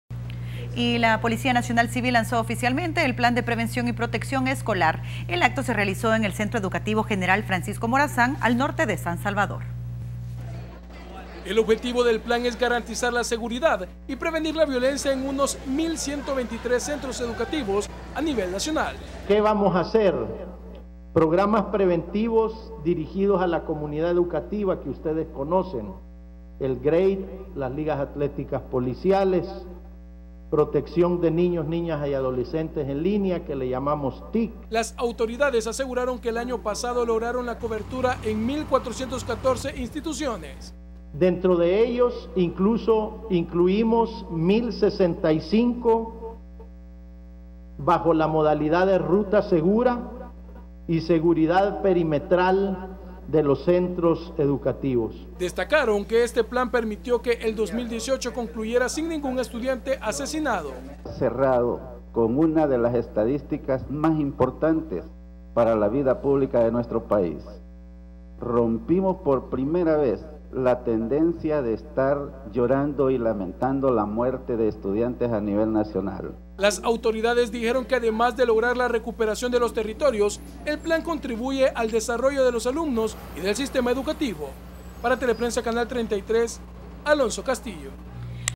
Lanzan plan de prevención y protección escolar
La policía nacional civil lanzo oficialmente el plan de prevención y protección escolar.

El acto se realizó en el centro educativo general francisco Morazán, al norte de San Salvador.